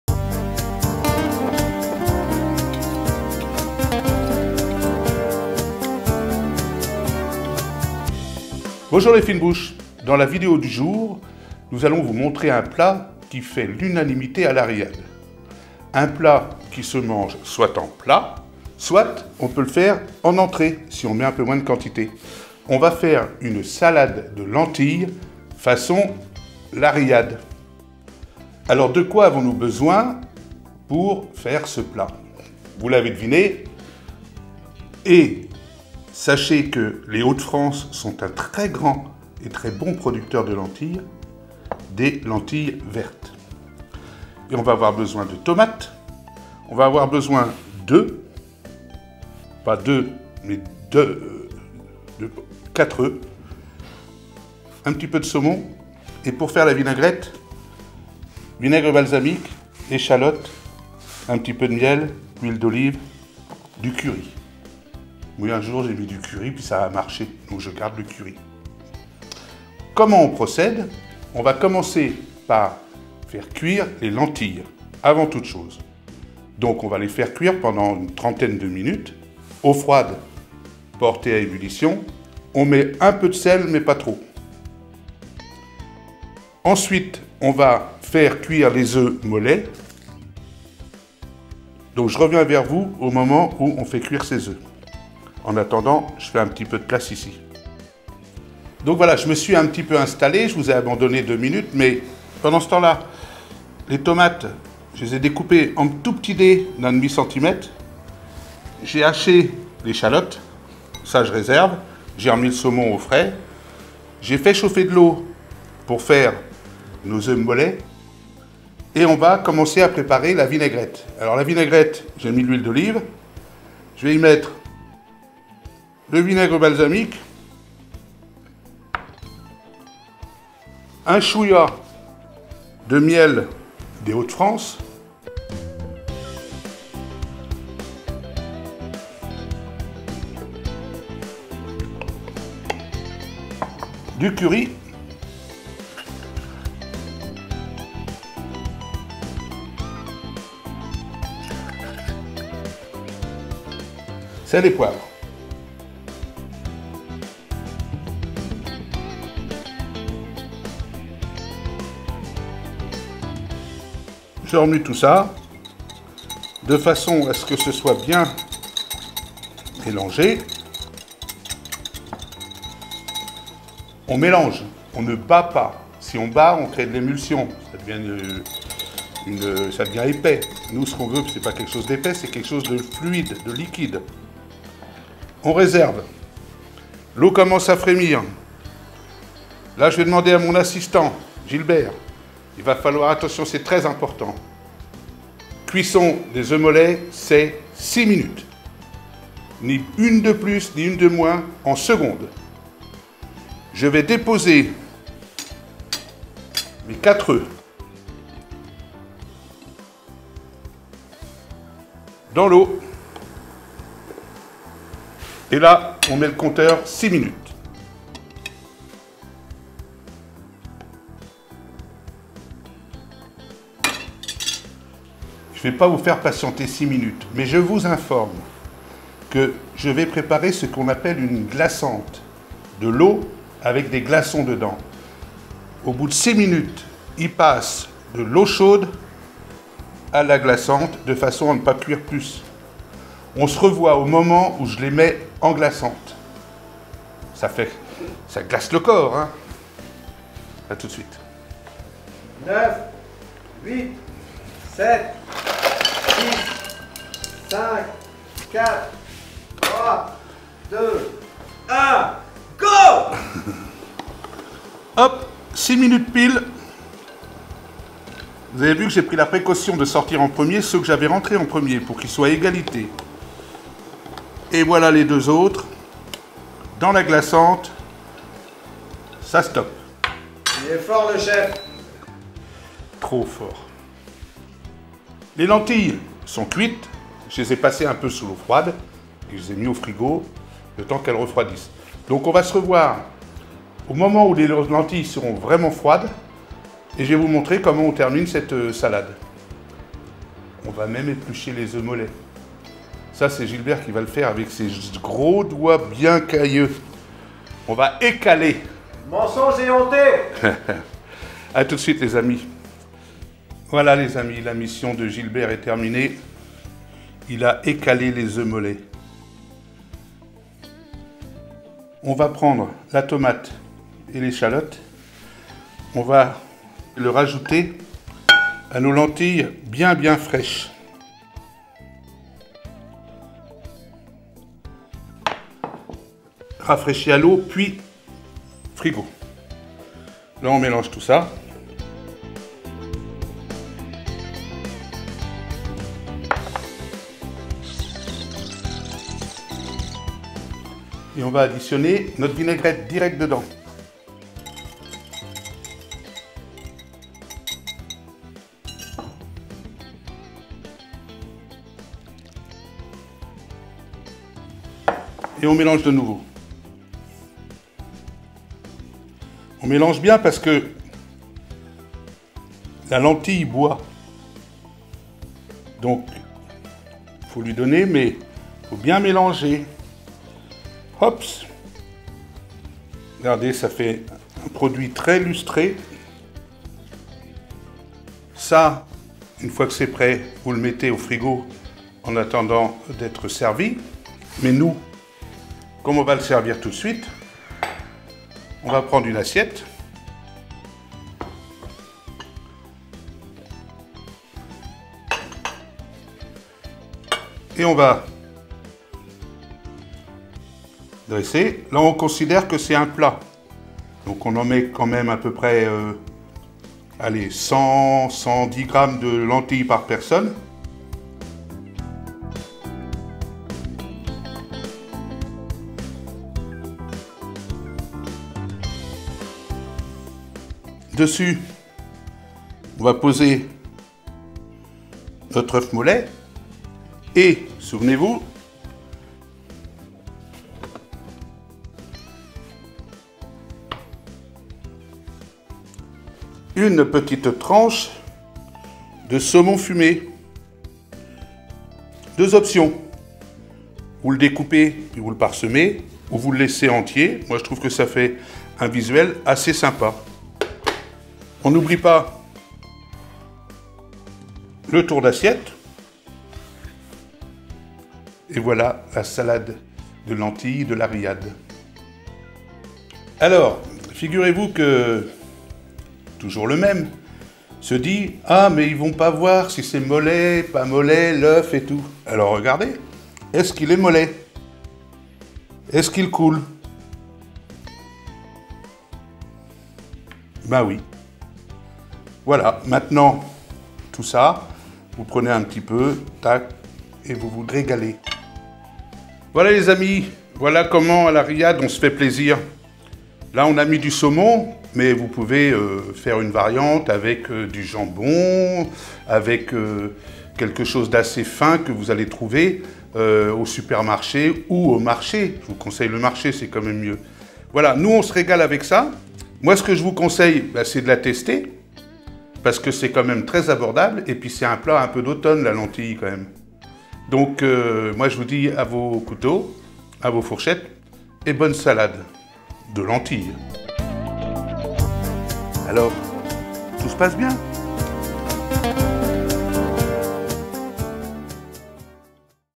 Salade de lentilles “La Riade“
Dans cette vidéo, comment réaliser une Salade de lentilles “La Riade“

saladedelentilles “La Riade“

Patrick Dujancourt
Licence